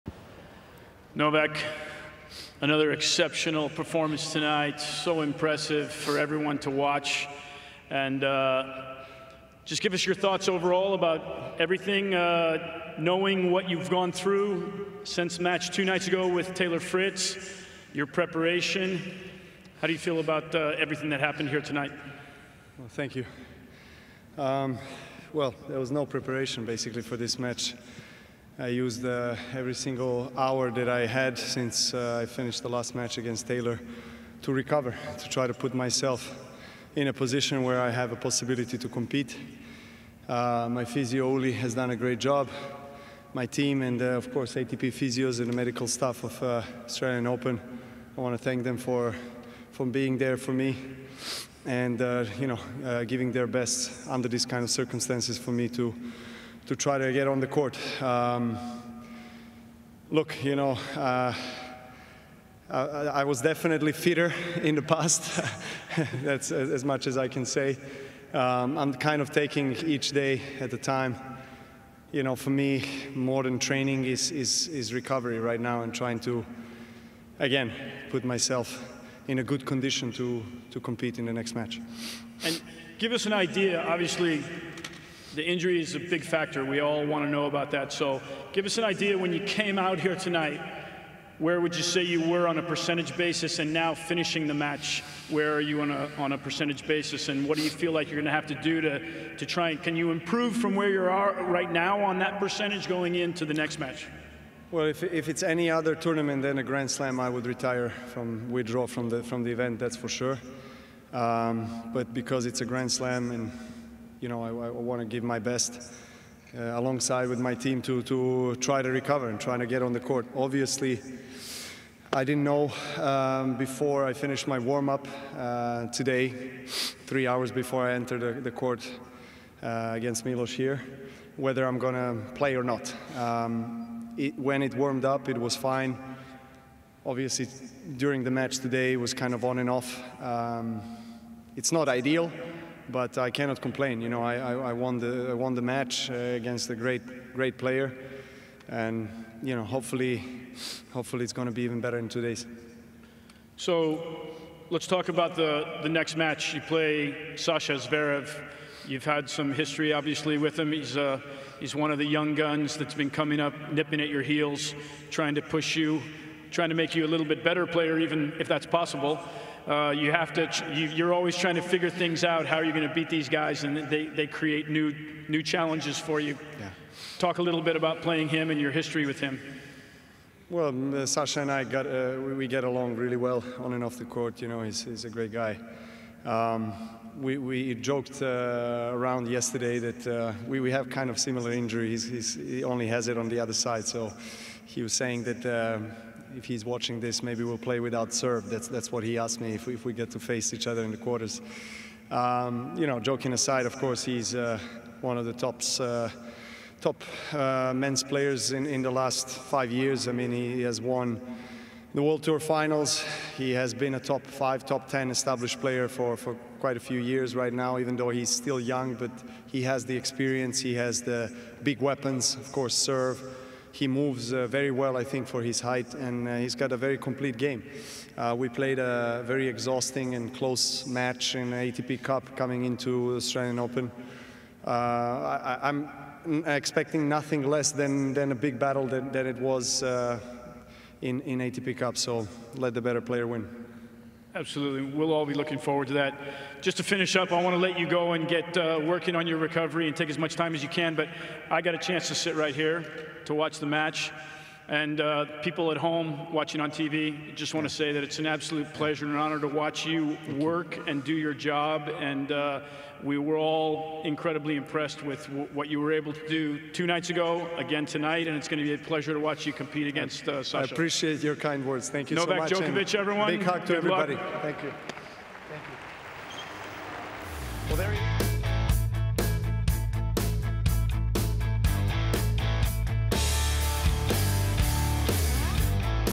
Novak’s startling admission following four-set win | Wide World of Sports
Novak Djokovic speaks after his fourth round win over Milos Raonic.